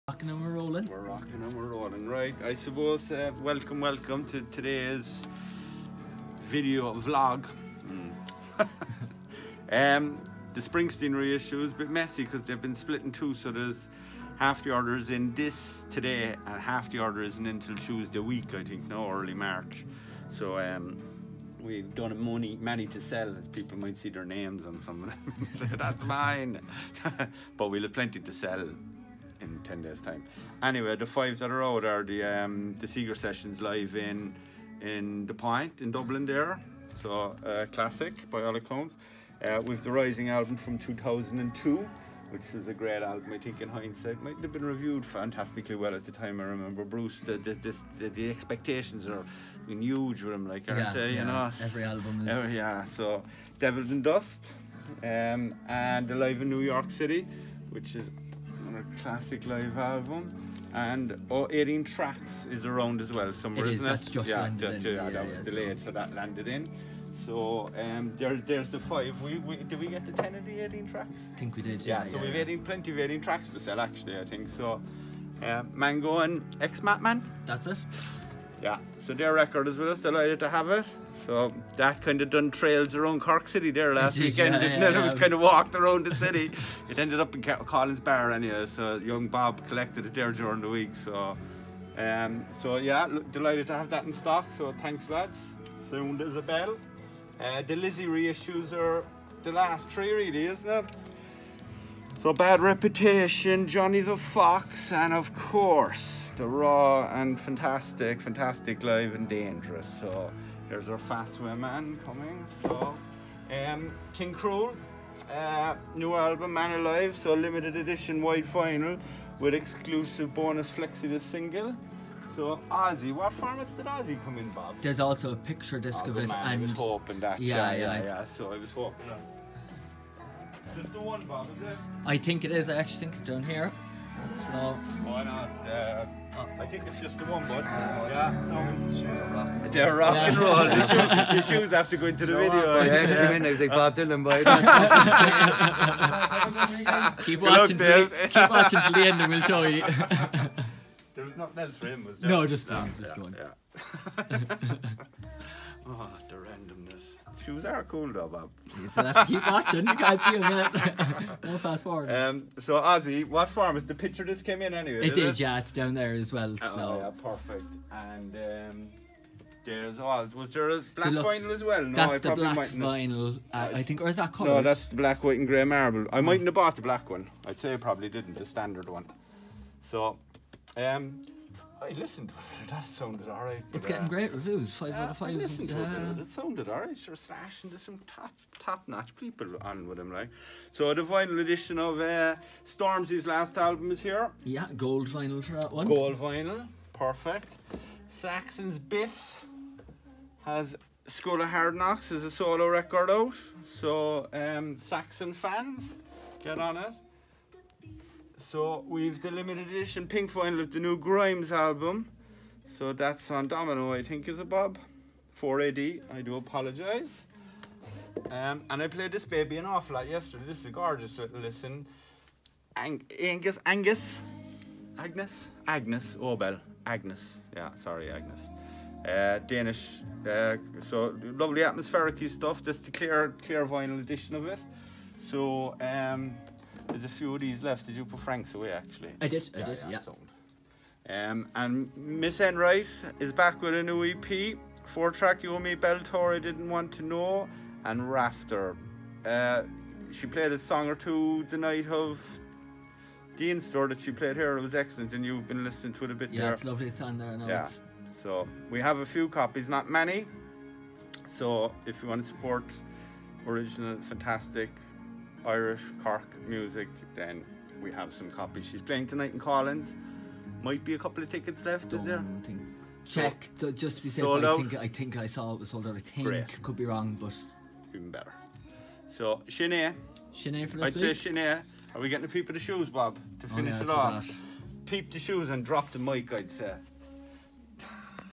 Music Zone : New Vinyl Record Releases February 21st 2020
Welcome back to the Music Zone video blog, bringing you all the latest releases in the world of vinyl records. Releases for this week include releases from Thin Lizzy, Ozzy Osbourne, Bruce Springsteen, Marlene Enright, and many many more.

To check availability of stock, pop instore to us at Unit 4, Deanrock Business Park, Togher, Cork , or check out our online store .